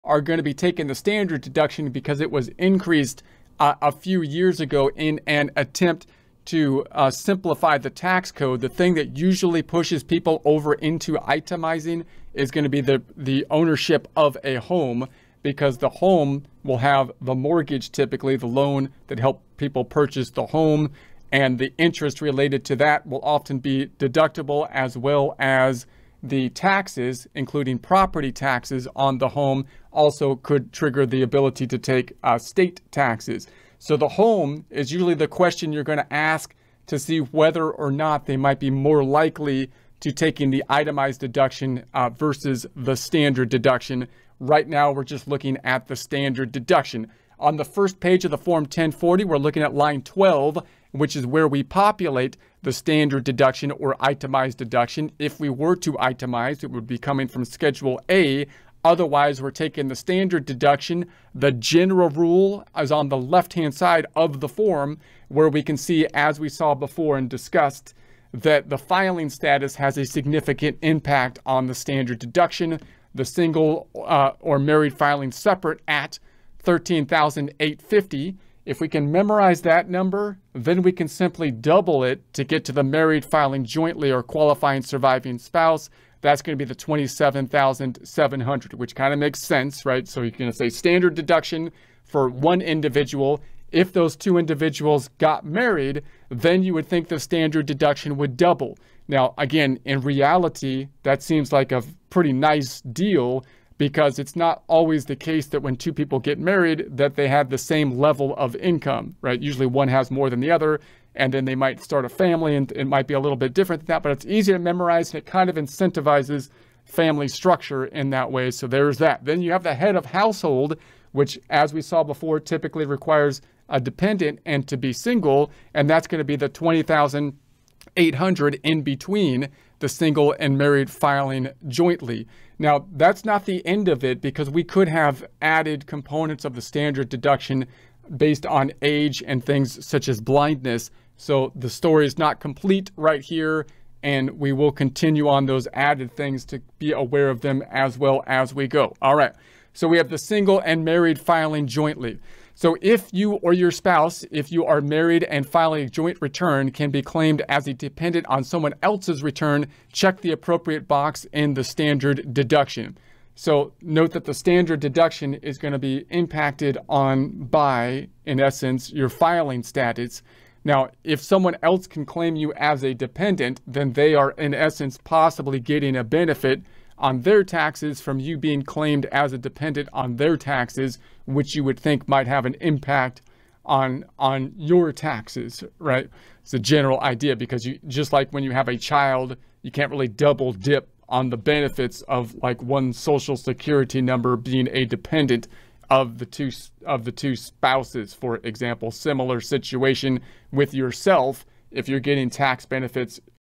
Filing Status and Standard Deduction Tax Preparation 2023-2024
📑 Unravel the secrets of Filing Status and Standard Deduction with our quick guide! 💼 Whether you're single, married, or somewhere in between, understanding your filing status and standard deduction is key to optimizing your tax situation.

🔍 In this video, we break down:
🔹 Different filing statuses and which one suits your situation.
🔹 How your filing status affects your tax liability.
🔹 The importance of choosing the right status for your financial scenario.
🔹 Quick tips on maximizing your standard deduction for ultimate tax savings.

💰 Don't miss out on potential refunds – empower yourself with the knowledge to make informed decisions during tax season! 🚀